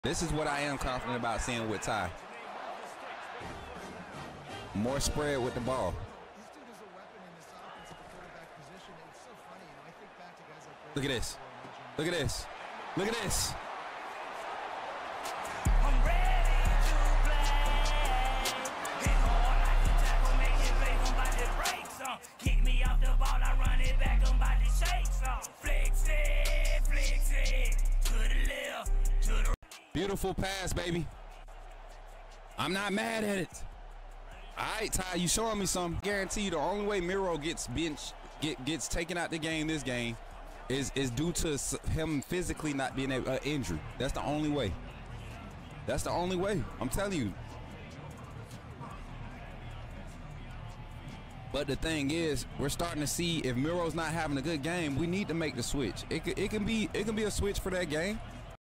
Can We See This From Ty Simpson ? Coach Smook Made the Move Live Game Sim !
We dive into Ty's incredible game, highlighting his impressive passes and skills. With Miro struggling, it's crucial to explore strategic substitutions. Watch to see how Ty is redefining his career and impacting the team's performance!